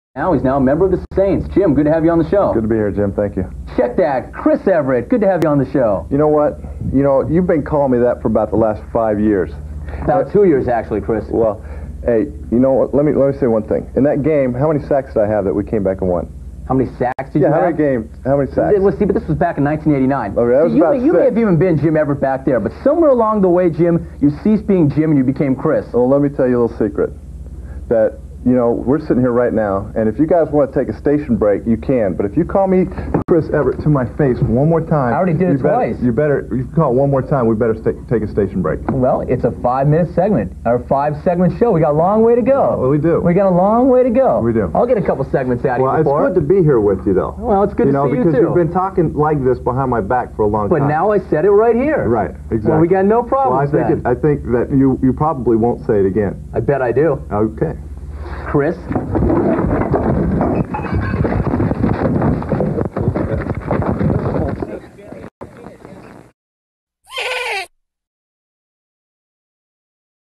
Jim Rome vs Jim Everett
One of the proudest moments of my (Deb Vogel) broadcasting career--
I was the producer (and smarty pants lower third provider) during this infamous ESPN broadcast from the last century
Deb